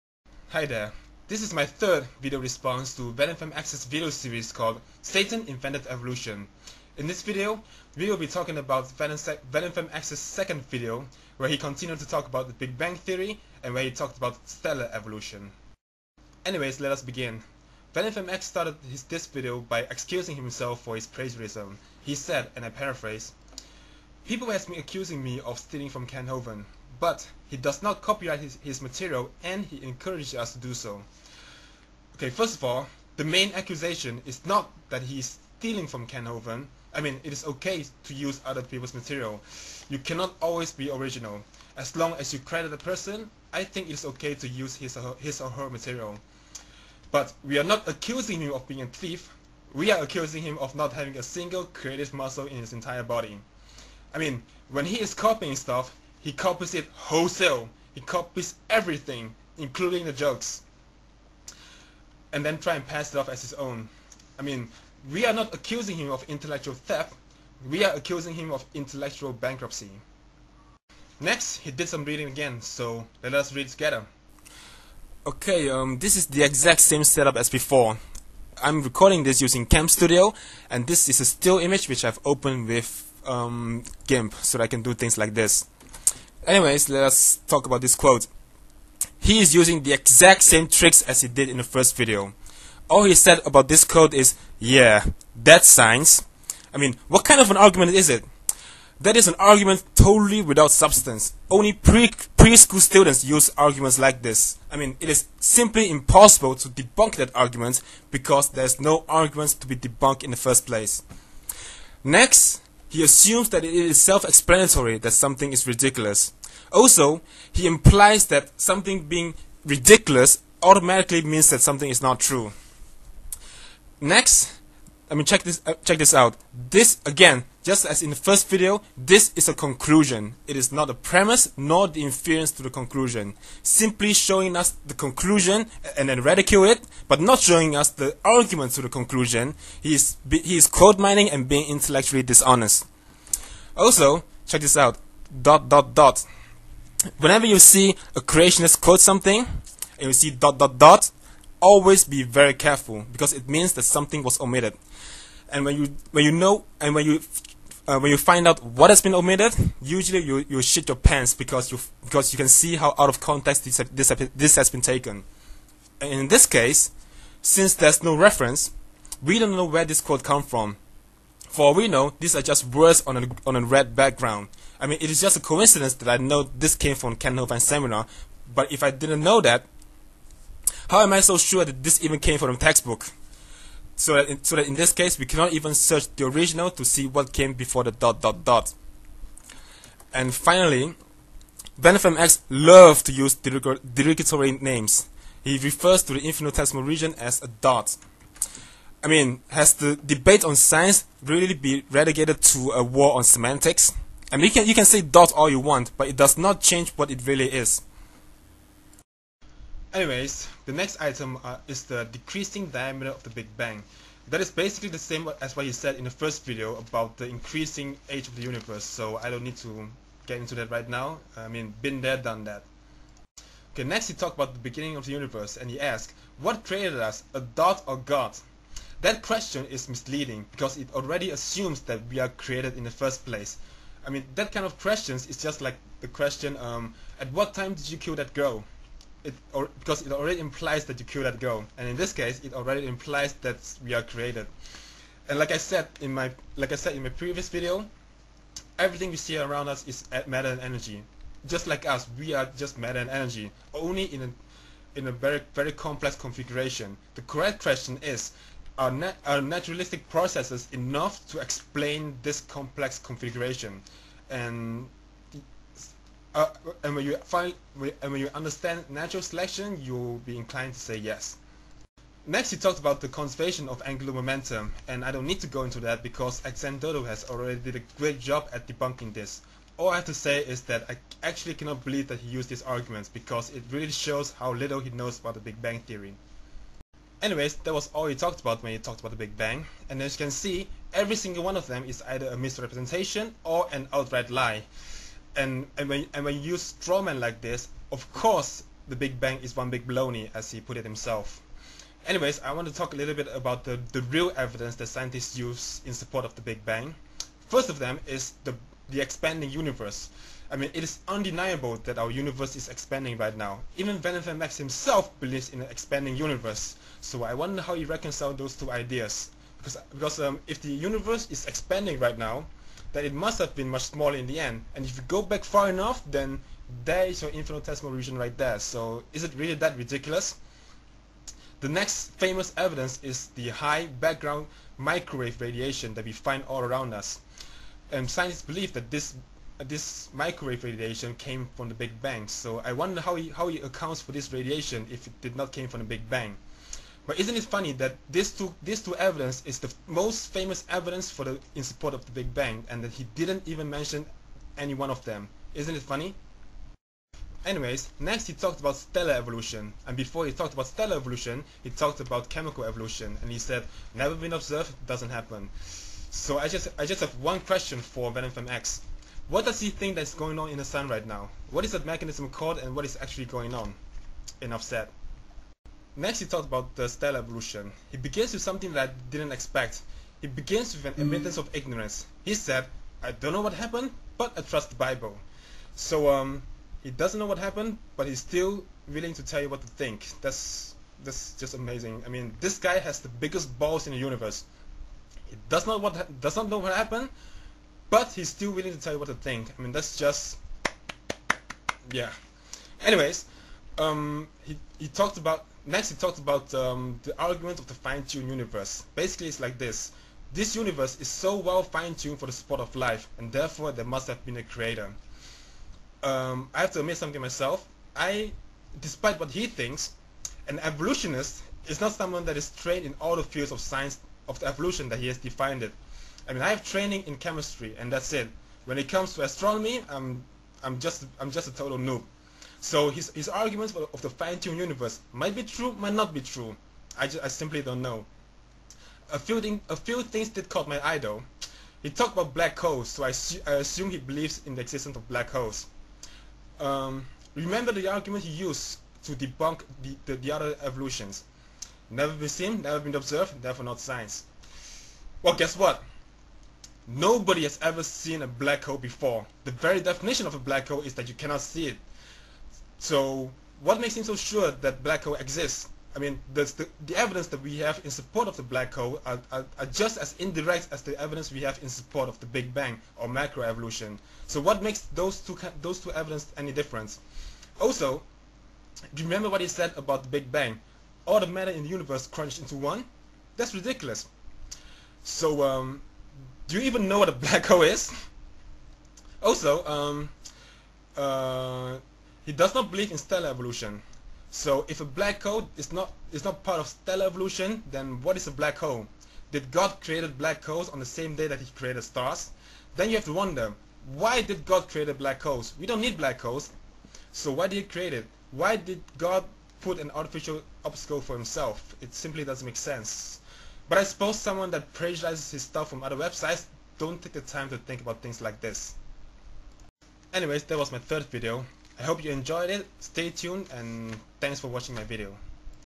Re: Satan Invented Evolution Pt 3: Stellar Evolution
The 'Who created us' question btw is a fallacy called the fallacy of presupposition.

I know I am kinda harsh to VFX in this video, but I can't hide my contempt for him anymore.